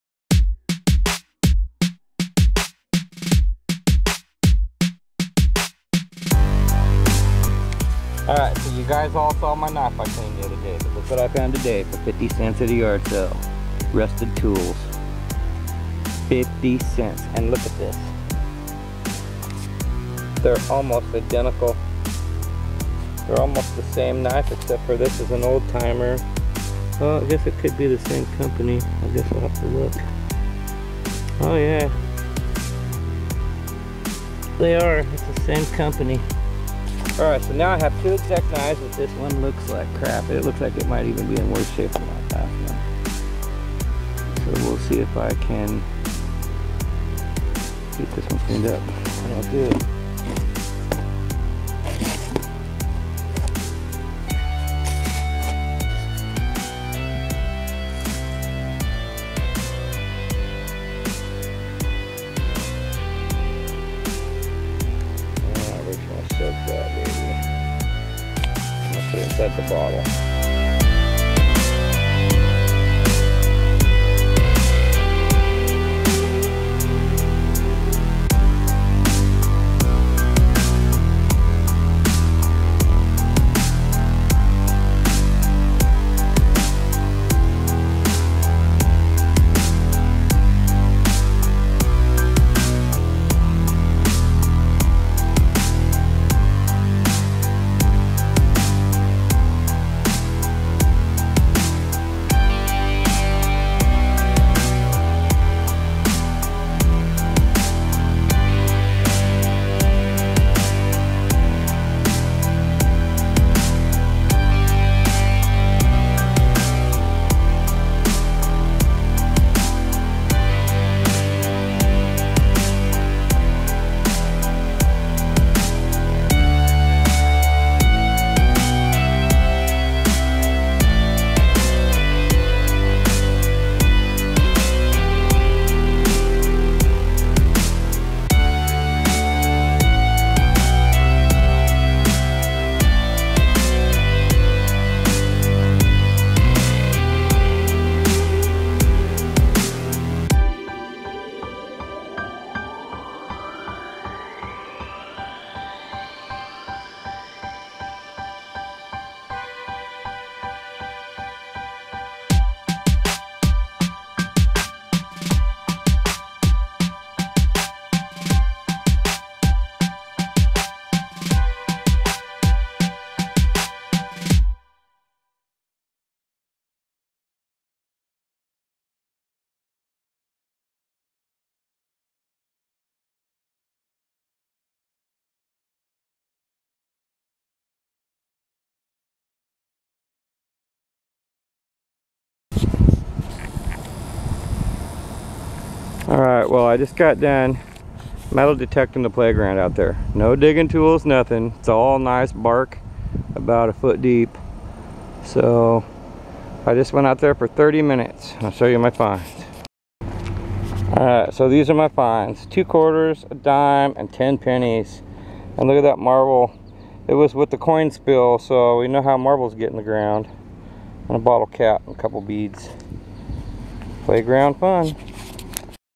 How -To Knife Restoration #2: Yard Sale Treasure (bonus clip and clue)
My luck is good this month. A second knife worth keeping I found at a yard sale for $.50. This Yard sale treasure cleans up nice. A short metal detecting video and ' marble clue' at the end! (Remember those clues)